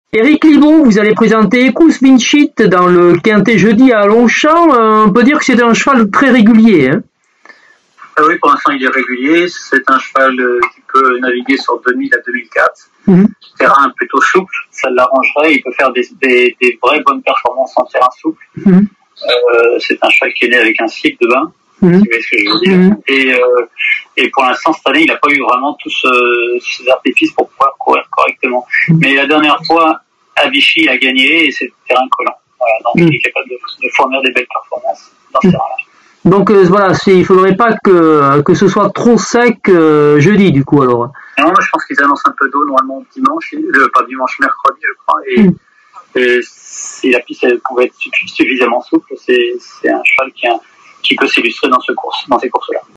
Eric Libaud, entraîneur de Equus Vincit (15/09 à ParisLongchamp)
Eric Libaud, entraîneur de Equus Vincit (10) nous parle de ses chances dans le Quinté+, le Prix de la Table sur l’hippodrome de ParisLongchamp le 15/09/2022.